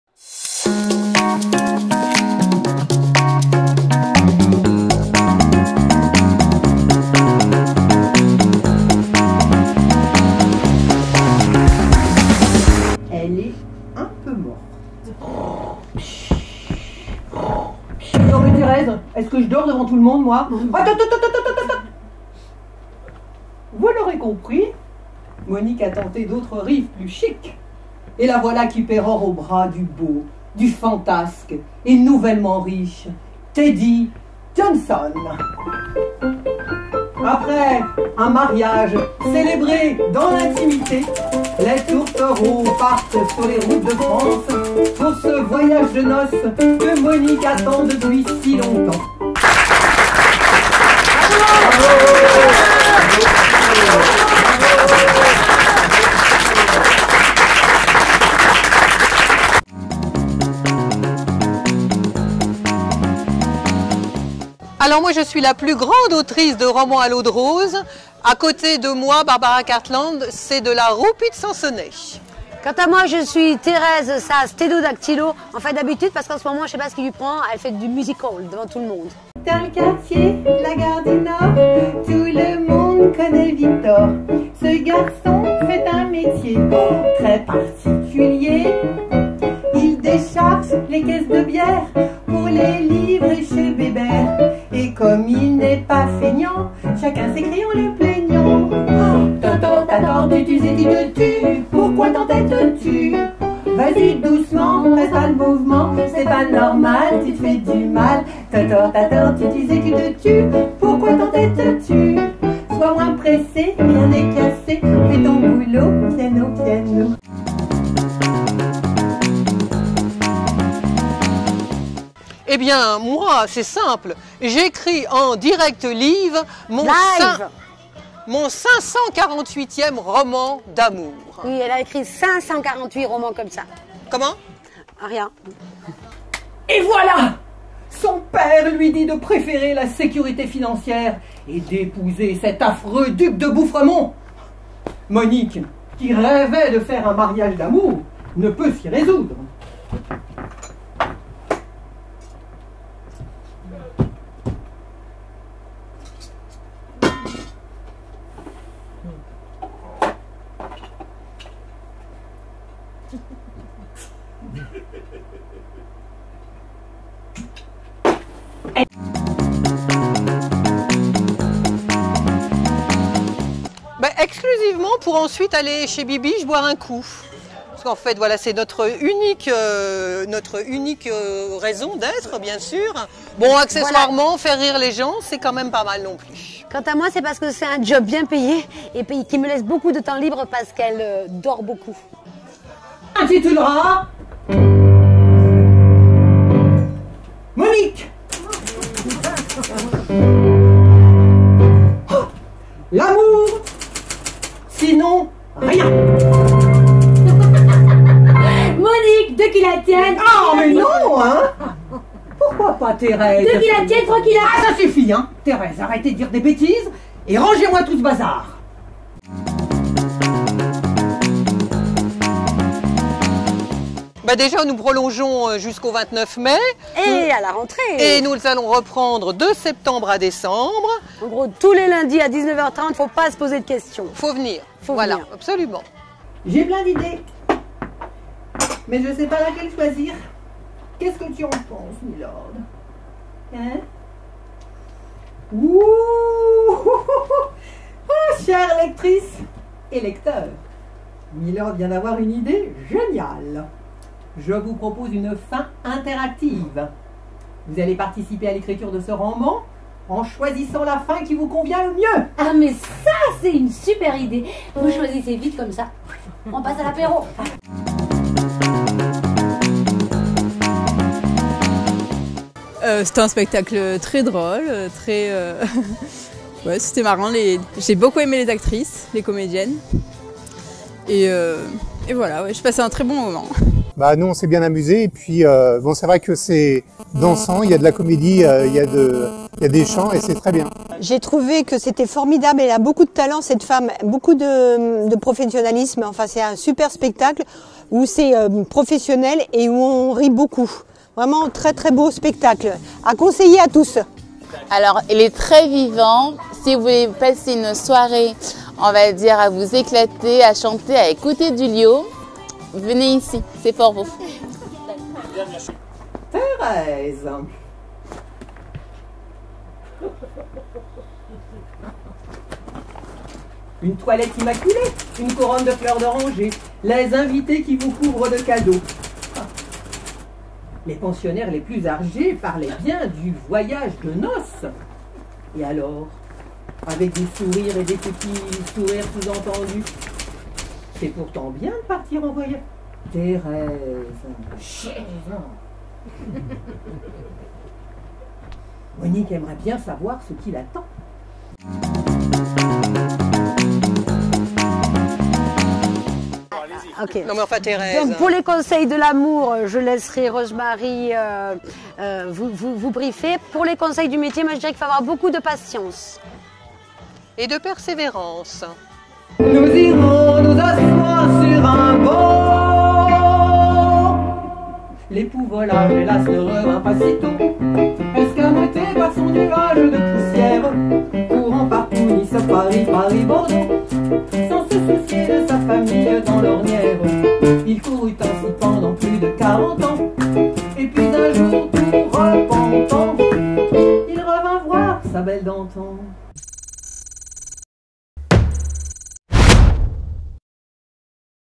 Rosemary Lovelace fait ça devant tout le monde au théâtre Montmartre Galabru
Auteur : Marie CHARLET accompagnée par Anne CADILHAC
Mise en scène : Frantz MOREL A L’HUISSIER
Interprètes : Marie CHARLET accompagnée par Anne CADILHAC

Rosemary Lovelace, grande prêtresse des romans à l’eau de rose, vous donne rendez vous pour assister à une performance inédite : l’écriture en directe « liiiiive » de son 548ème roman dont le titre vous sera dévoilé lors de la soirée !
Flanquée de Thérèse, sa femme de chambre, assistante couteau suisse, qui l’accompagnera aussi au piano, elle déroulera son éternelle recette : une héroïne un peu oie blanche à la recherche de l’homme idéal ! Parcours du combattant s’il en est pour la jeune fille, qui au gré d’aventures rocambolesques, découvrira les hommes, les torrides nuits d’amour, les joies et les déboires qui font le sel de la vie.
Le récit est ponctué d’airs coquins et fantaisistes majoritairement issus du répertoire des années 20-30 (Mistinguett, Charles Trenet, Colette Renard…) avec quelques clins d’œil à Boris Vian, Lio et Dalida. Et comme Rosemary Lovelace veut rester « in », elle proposera au public une fin très interactive.
Ne croyez pas les journaux à scandale qui la brocardent : « La vieille rose » en a encore sous le capot !
0:00 Intro
00:59 Présentez vous succinctement
01:58 Présentez votre spectacle
02:49 Pourquoi avez vous monté ce spectacle?
03:39 Votre actualité
04:28 Réactions du public
5:51 Conseil à la jeune génération